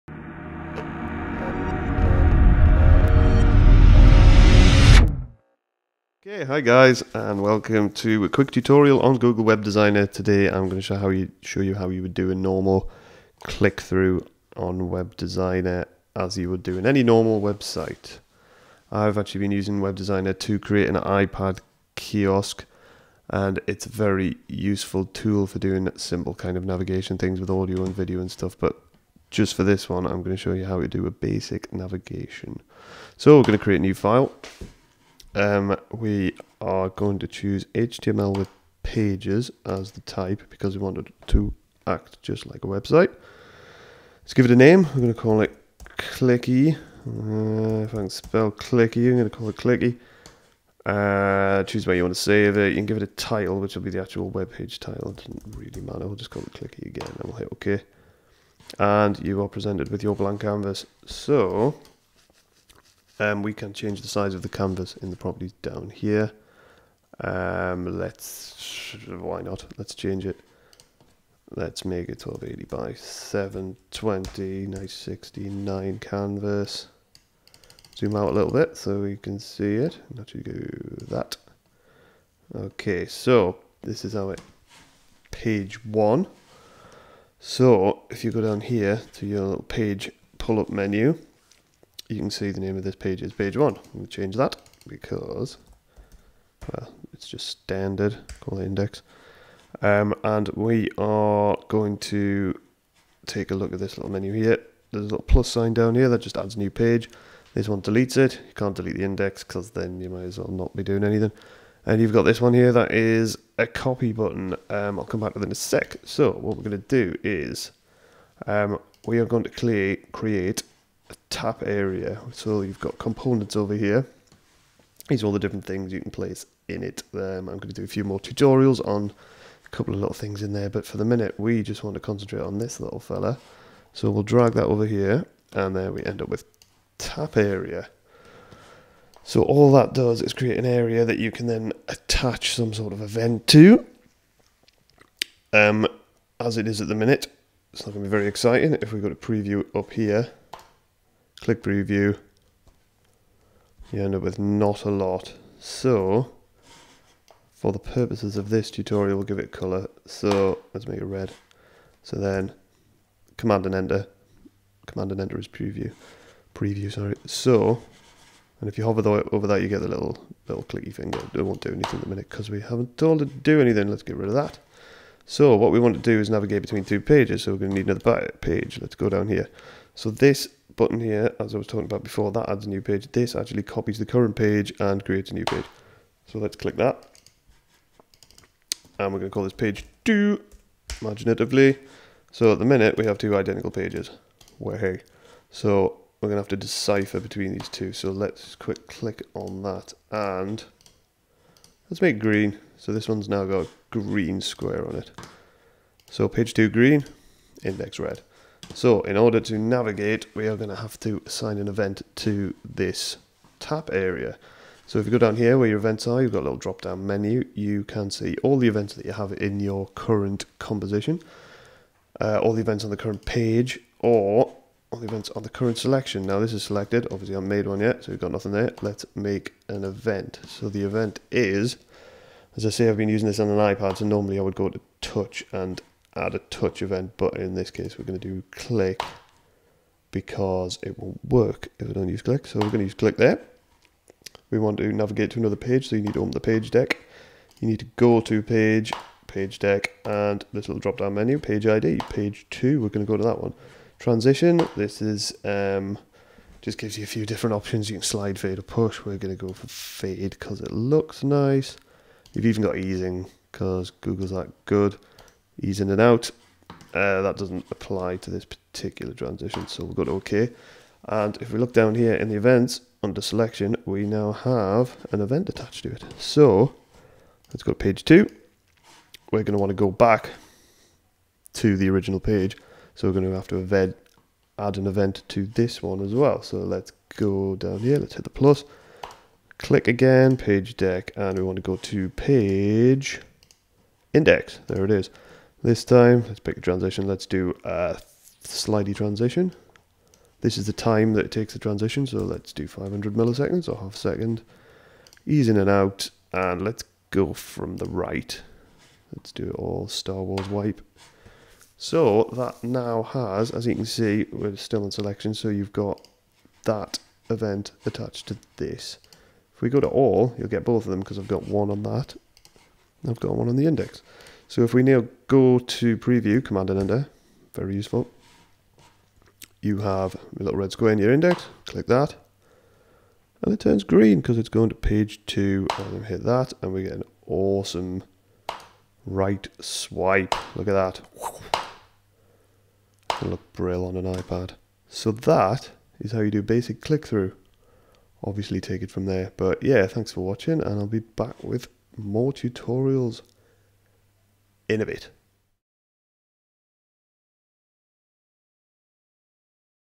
Google Web Designer Click Through Tutorial
A quick navigation tutorial for Web Designer, showing how to switch between pages like a normal web site